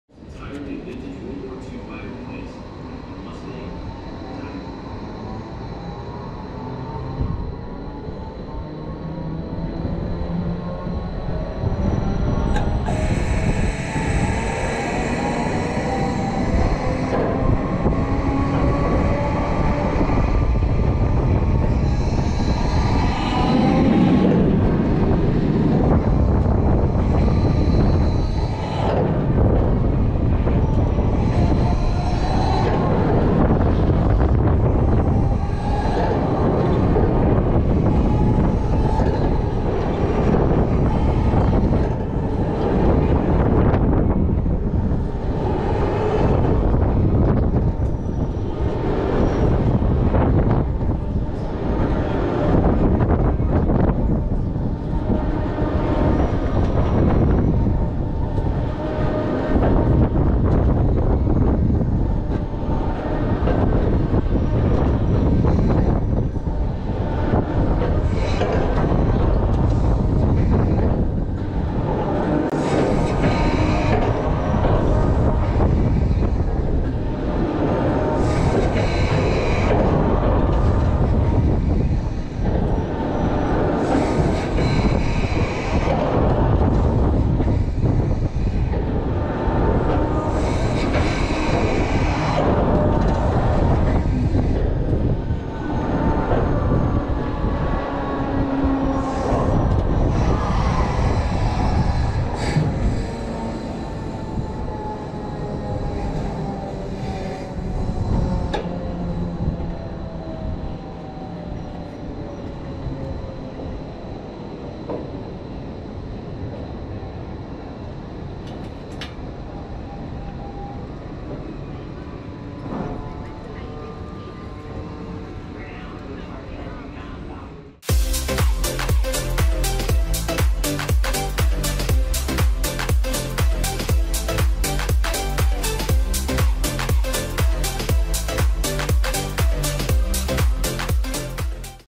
Air Race 4K On Ride POV - Drayton Manor
Take a ride on Air Race at Drayton Manor in our on ride POV. Manufactured by Zamperla, up to 4 riders can sit in each plane for a flight above Adventure Cove! As this thrilling ride spins, each plane rotates 360 degrees, swinging you upside down and towards the ground!

Please note, on ride filming is strictly prohibited at Drayton Manor.